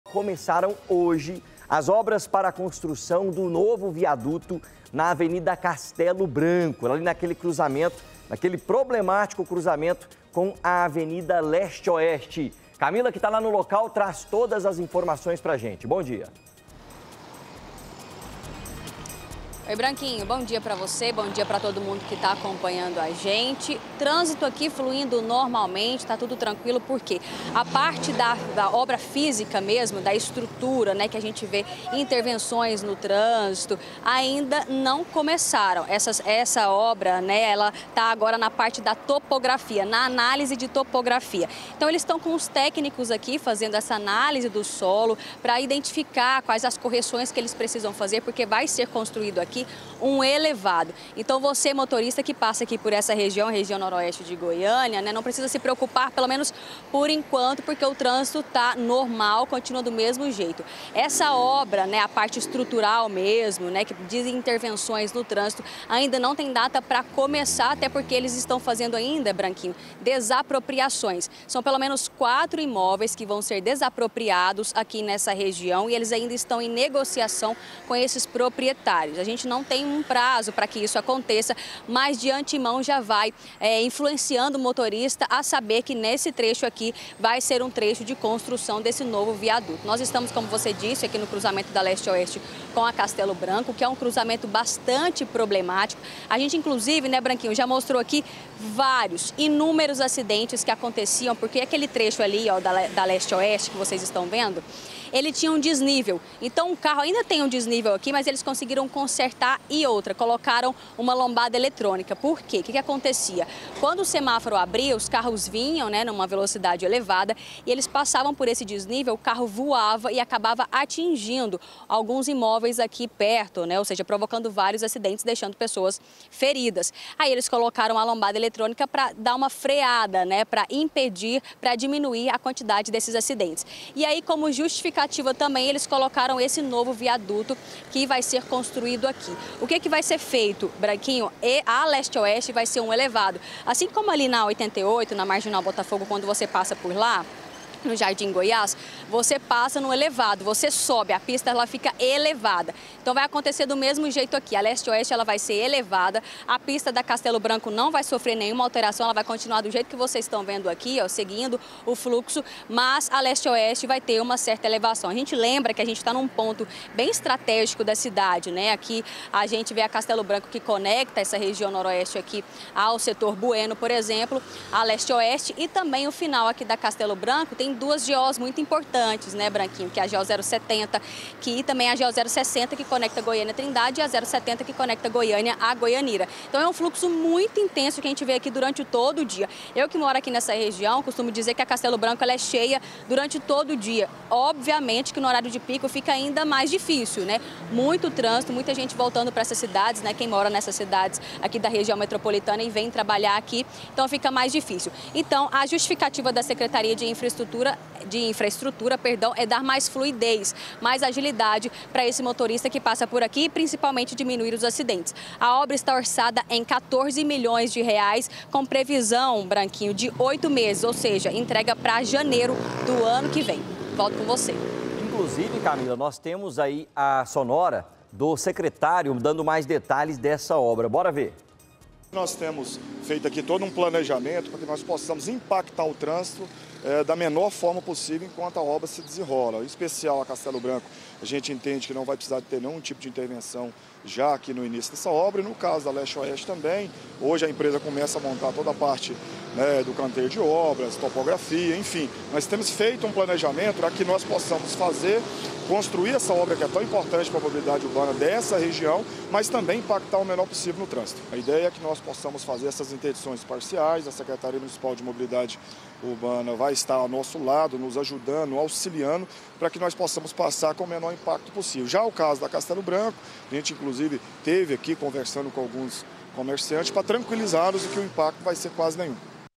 NOVO VIADUTO: CONSTRUÇÃO DO COMPLEXO VIÁRIO NA CASTELO BRANCO
O CRUZAMENTO DAS AVENIDAS LESTE OESTE E CASTELO BRANCO, EM GOIÂNIA, VAI GANHAR UM VIADUTO. AS OBRAS COMEÇARAM HOJE E DEVEM DURAR CERCA DE 8 MESES.